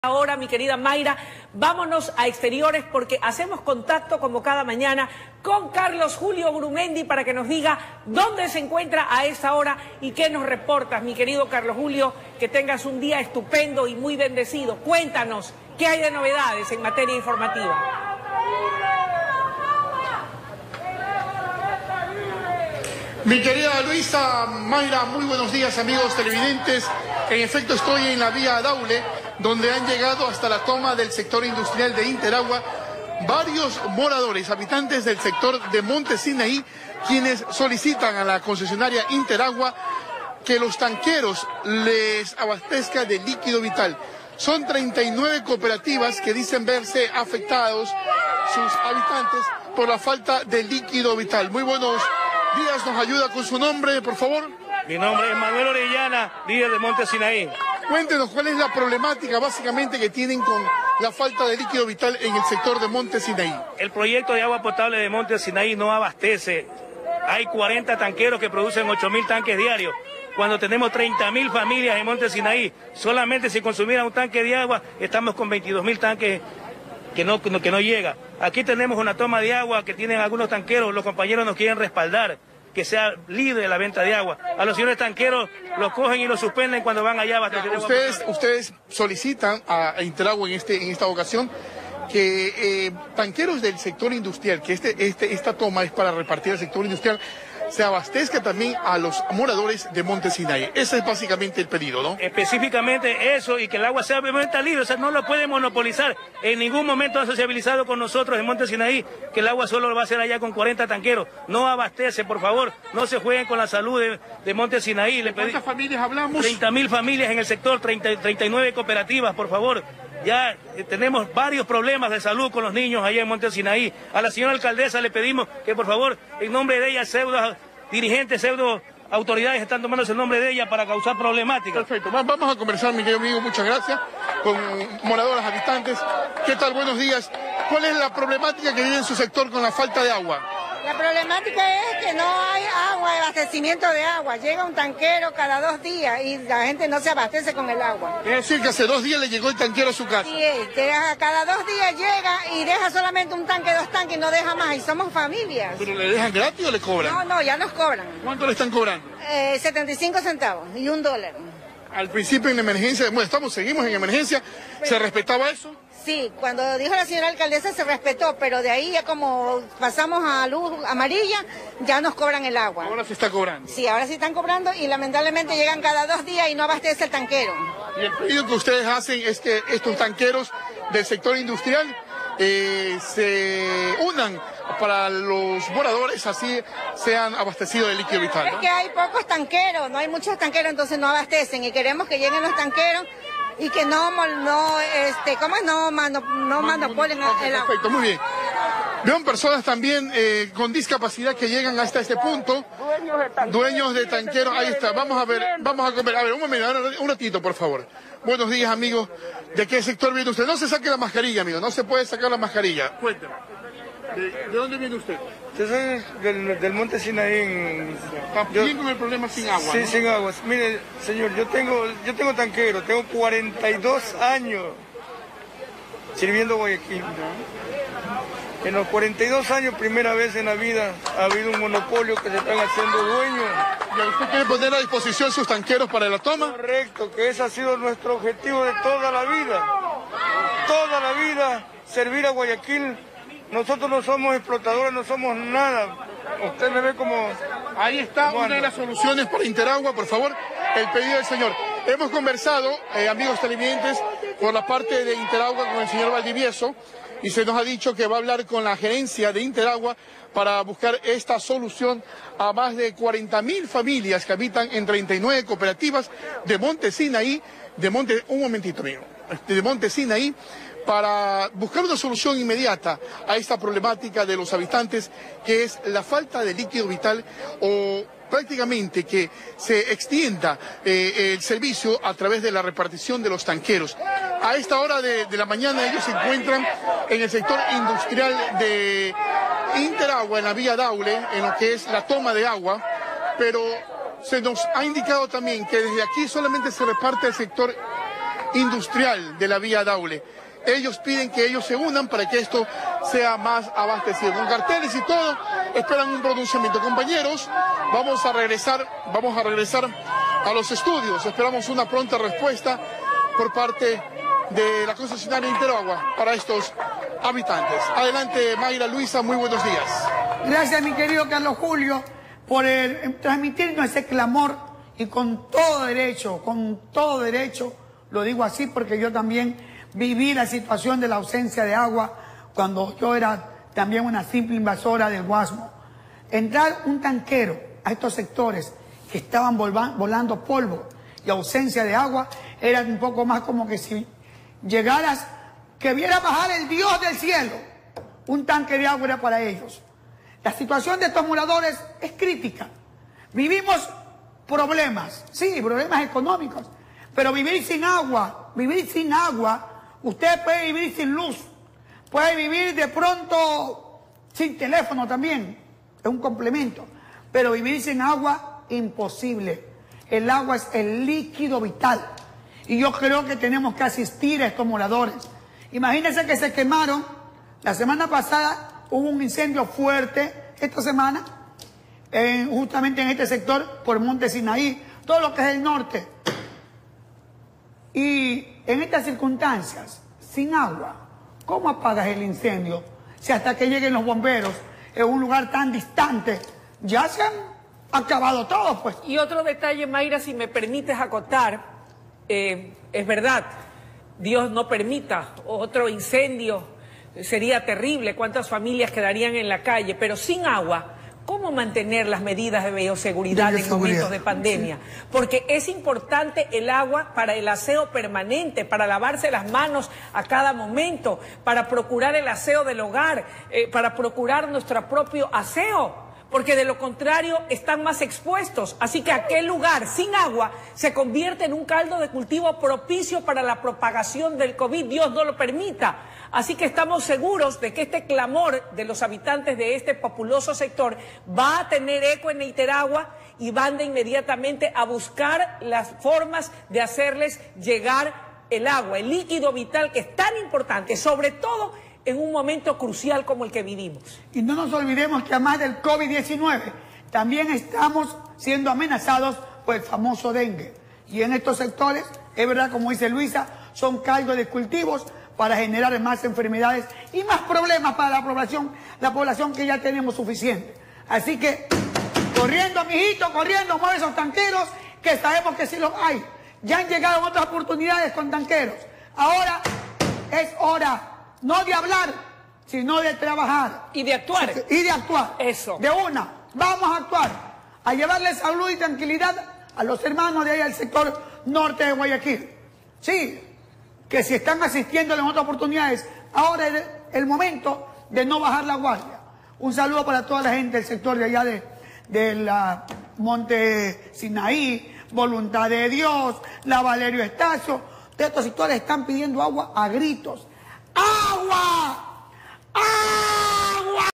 Moradores realizan una protesta por la falta del agua potable
Más de 39 de cooperativas de Monte Sinaí  se encuentran afectadas

Esta manifestación se lleva a cabo en las cercanías de la toma del sector industrial de la empresa Interagua.